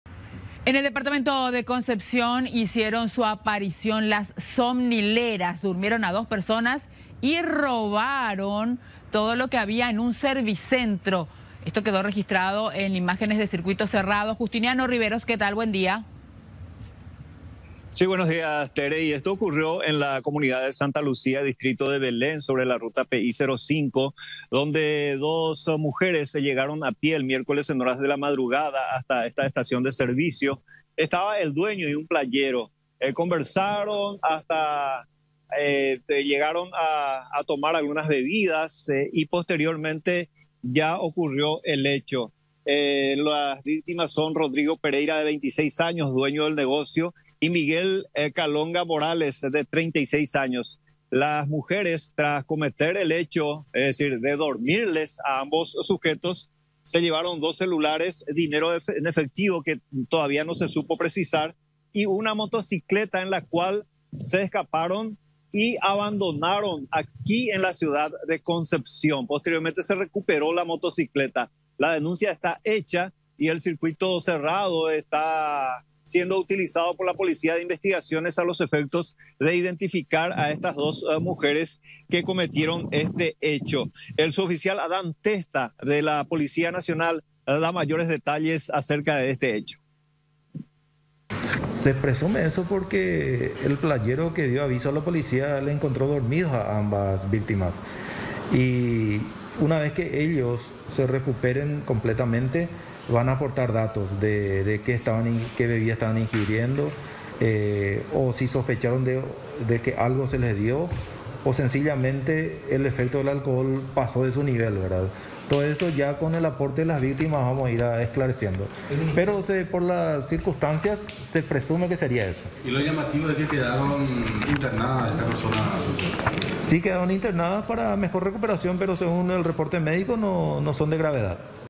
Somnileras robaron en una gasolinera de Concepción
Dos mujeres "durmieron" con somníferos a los trabajadores de una estación de servicios y robaron toda la recaudación del día. Luego del asalto huyeron a bordo de una motocicleta y hasta el momento, no fueron identificadas.